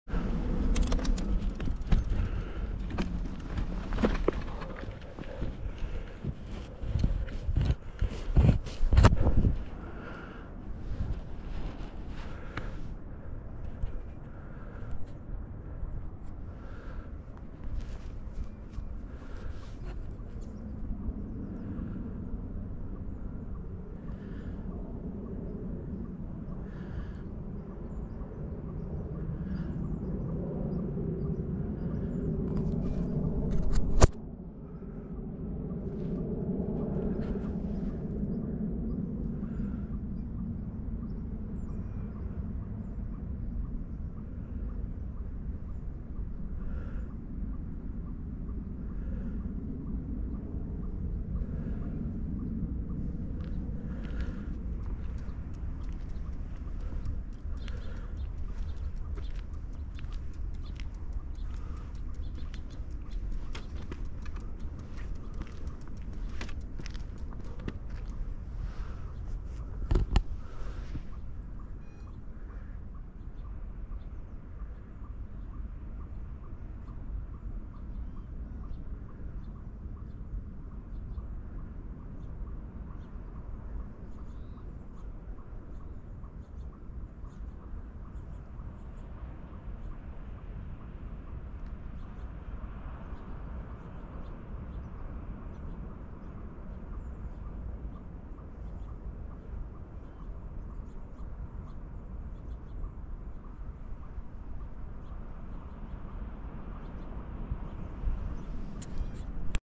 RedTail hawk 8-24-22
A Red-tailed Hawk on Bunker Road in Rodeo Valley, Marin Headlands, California.